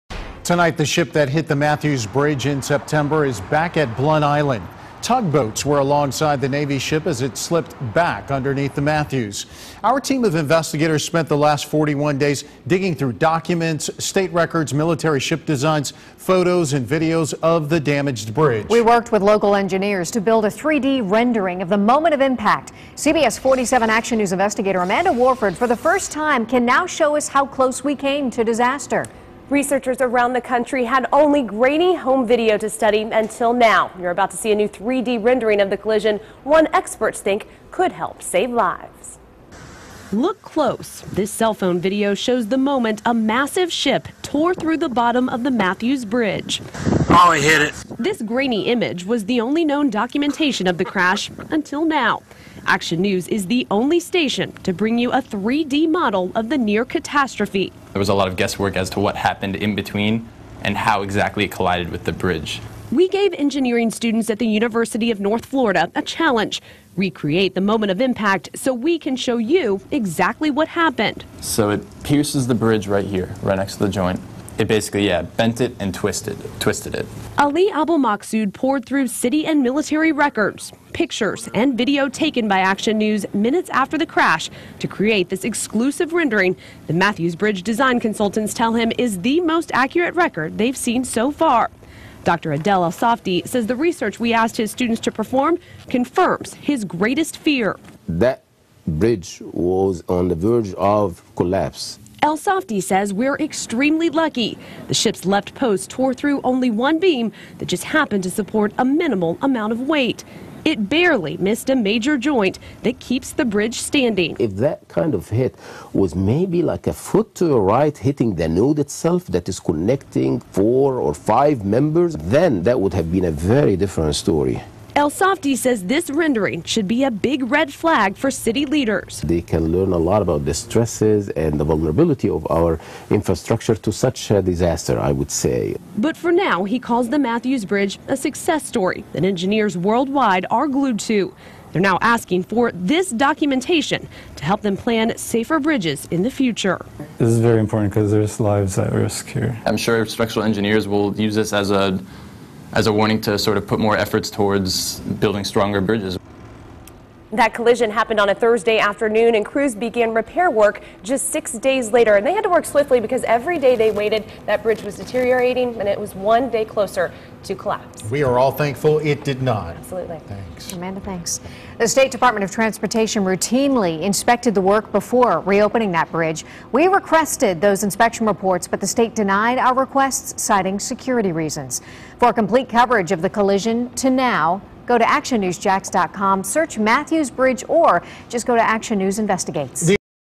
Exclusive Recreation of Mathews Bridge Collision Action News
Animation for Mathews Bridge Collision- Adel ElSafty & Ali Abdelmaksoud